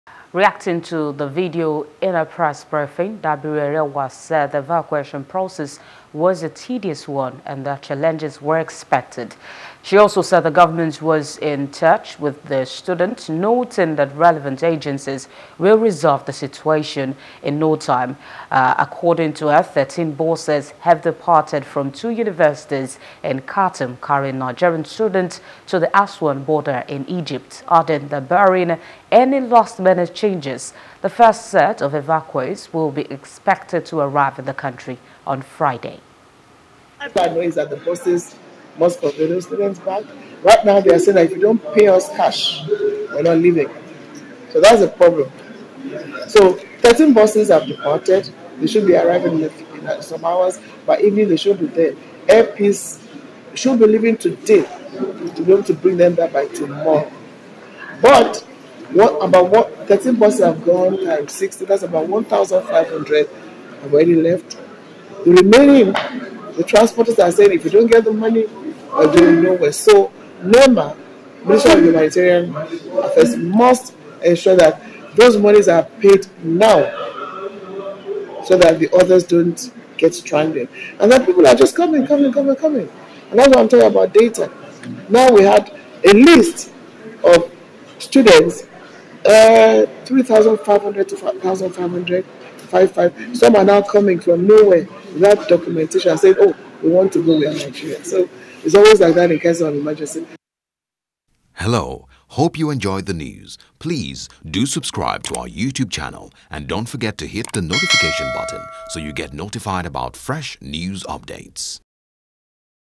Sudan Conflict: Federal Government Promises Stranded Nigerian Students In Desert Will Be Moved Out
Reacting to the video in a press briefing on Thursday, Dabiri-Erewa said the evacuation process was a tedious one and that challenges were expected.